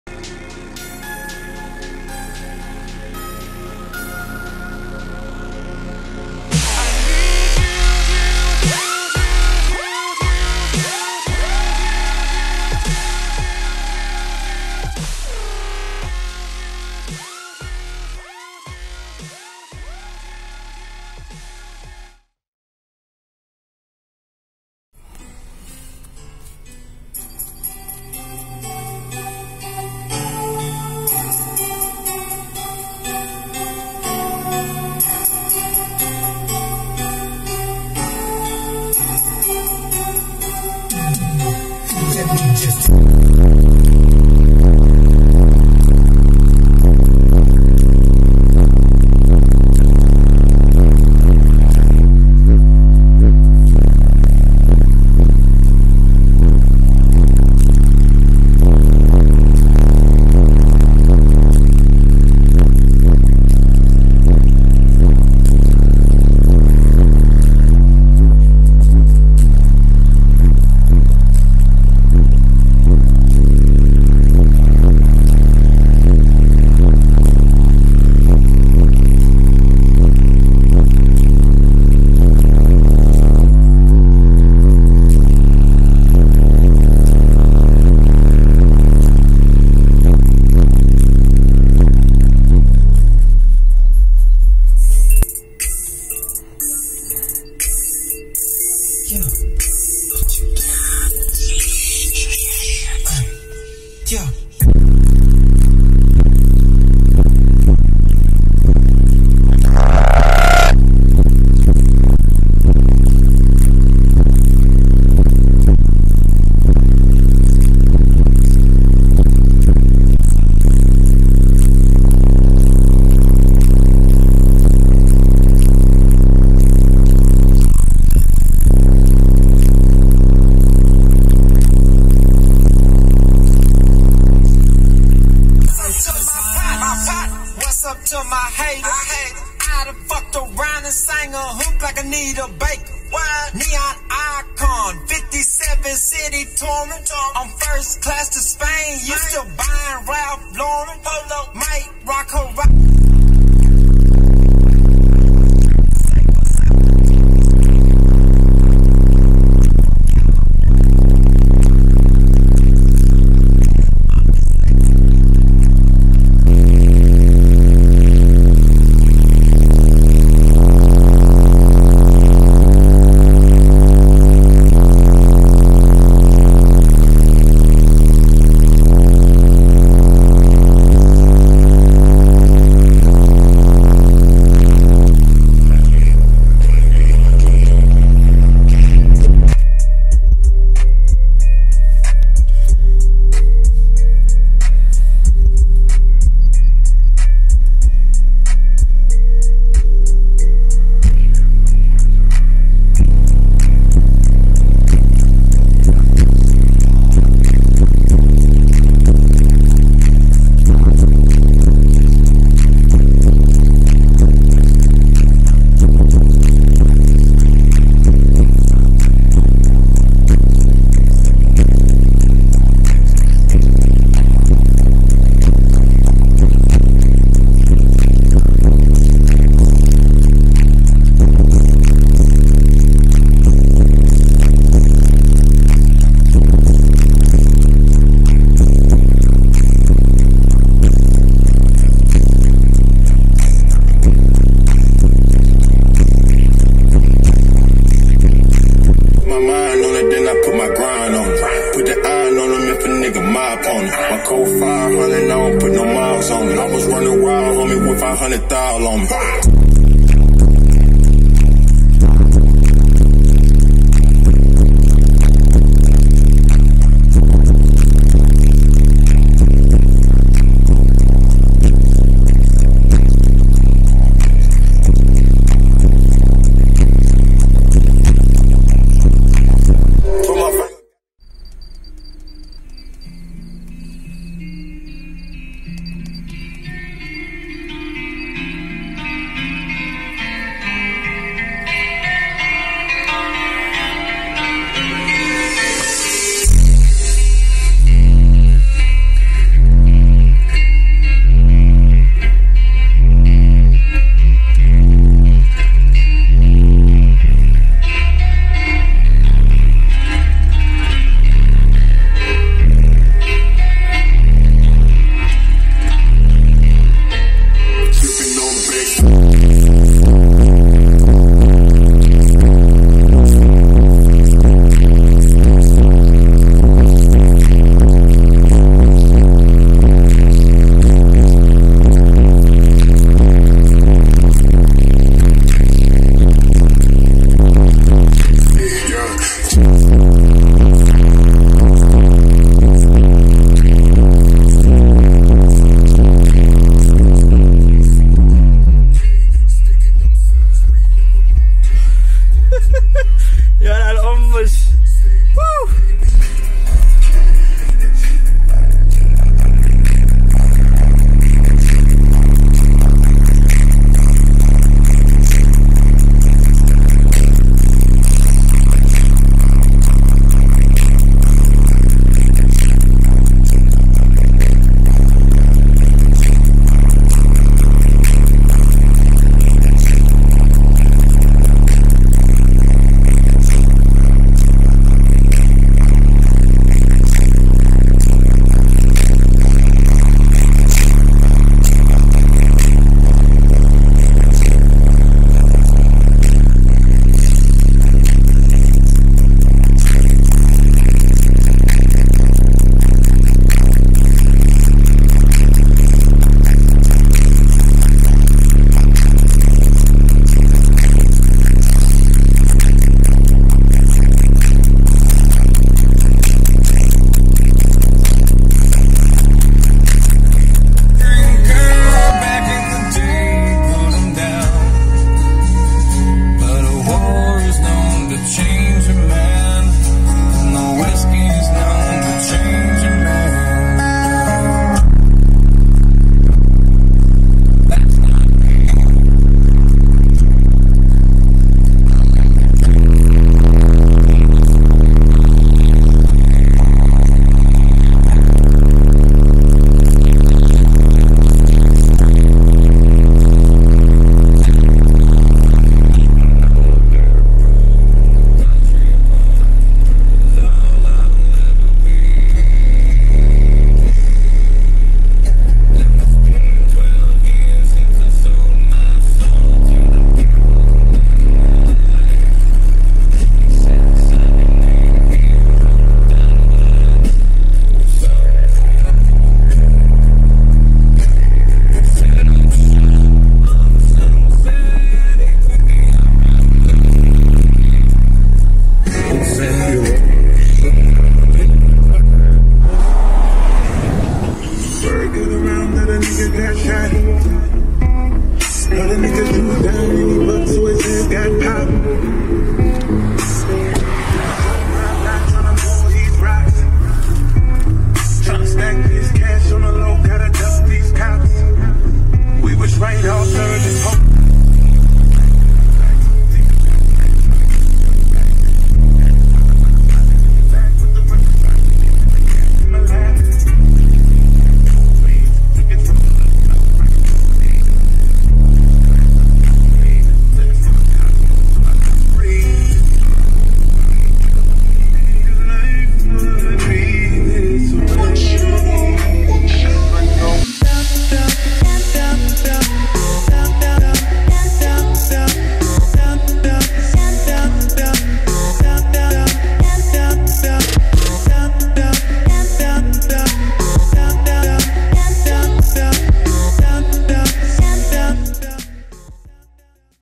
2009 Honda Civic Two 12" SKAR EVL | Hertz Millie 3 Way | ساوند سيستم على هوندا سيفيك
2009 HONDA CIVIC

Trunk Ported Enclosure

Pioneer AVH-X8750BT Headunit

DRIVERS:
(2) Skar Audio EVL 12” D4 Subwoofer

(1) Hert MPK Millie 163.3 (3-Way) Front component speaker
(1) Skar SPX-65C (2-Way) Rear-door component speaker
(4) Pioneer Coaxil 6x9 Rear deck speaker

AMPLIFIERS:
(1) Skar Audio SKv2-3500.1D   (Subwoofer)
(1) Skar Audio RP-150.4AB (Mids and Highs)

ELECTRICAL:
- Big 3 Upgrade
- OFC 0 Gauge cables

- Stock alternator